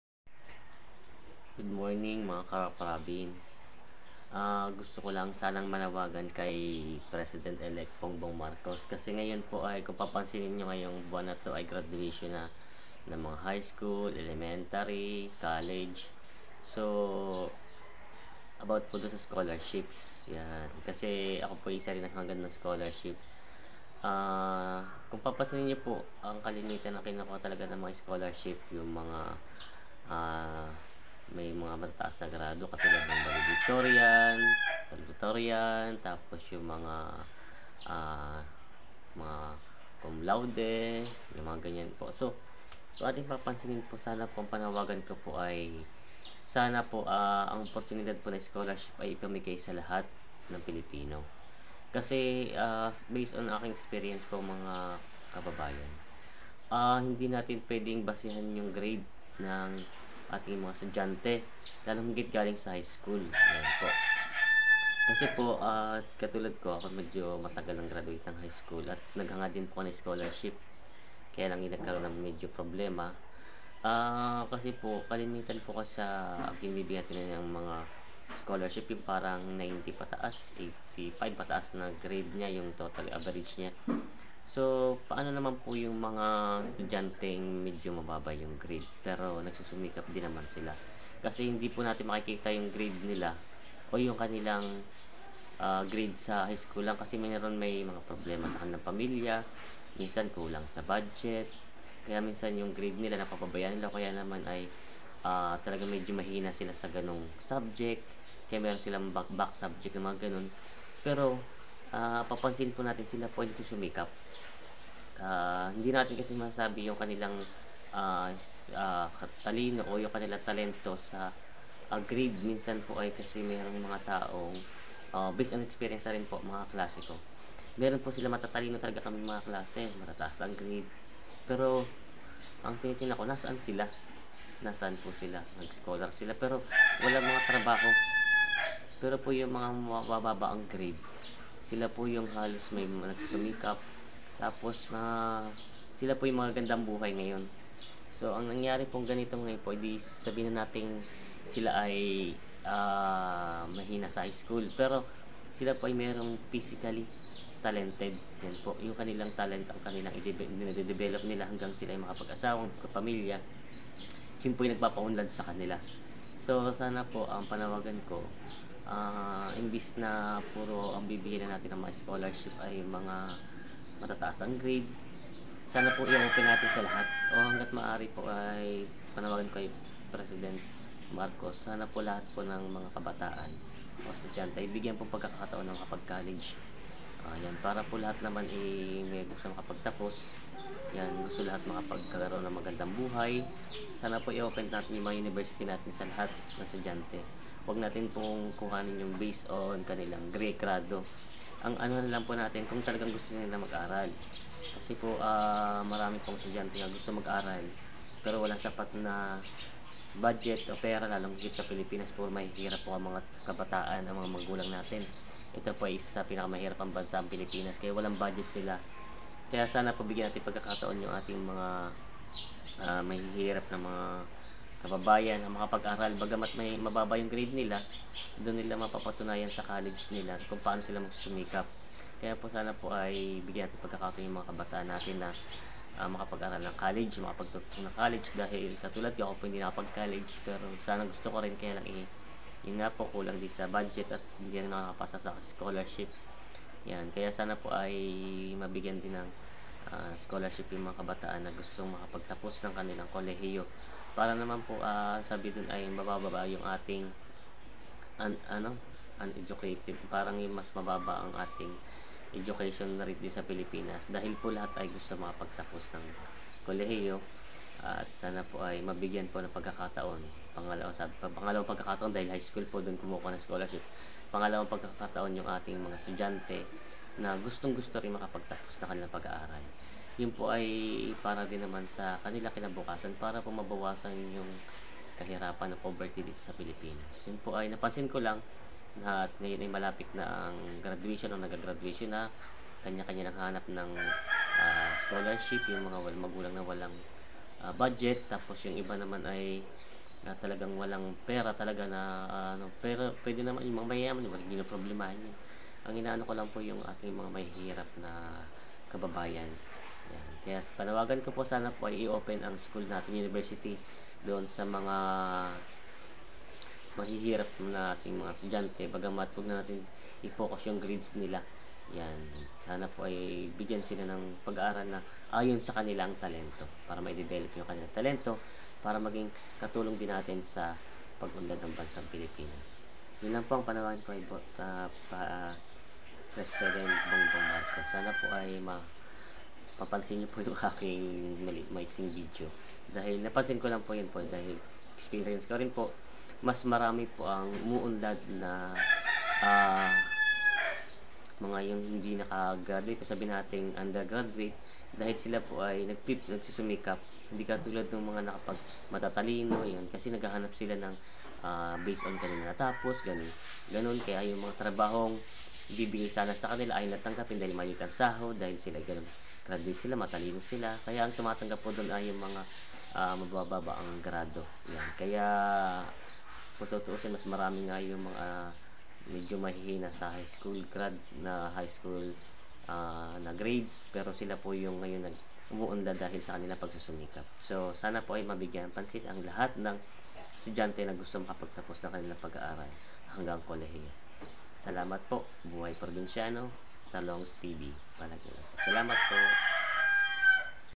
Panawagan ng isang magulang ngayon kay pres.BBM tungkol sa scholarship ngayon. Viral@talongstv
SCHOLARSHIP TO ALL PILIPINO PEOPLE